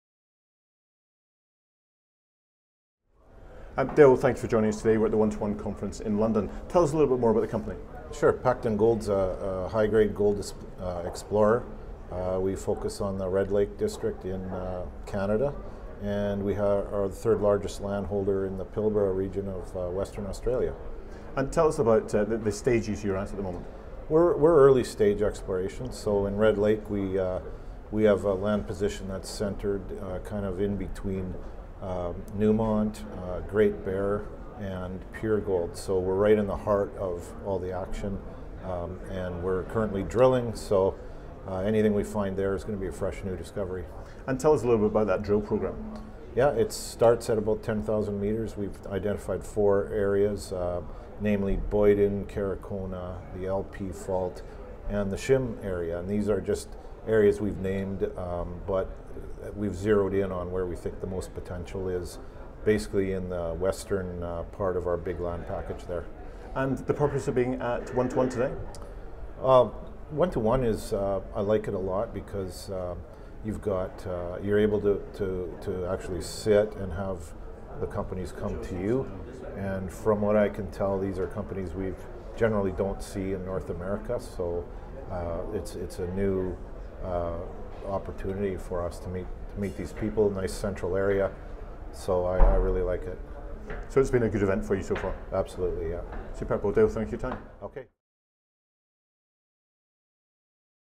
Interview: Dale Ginn, Pacton Gold - 121 Mining Investment London 2019 Autumn
Primary ticker: TSXV:PAC
Stage of development: Exploration
Primary minerals: Gold
Project to promote: Red Lake

Pacton Gold is a Canadian exploration company with key strategic partners focused on the exploration and development of high grade conglomerate and orogenic gold properties located in the district-scale Pilbara gold rush in Western Australia and the Red Lake District, Ontario.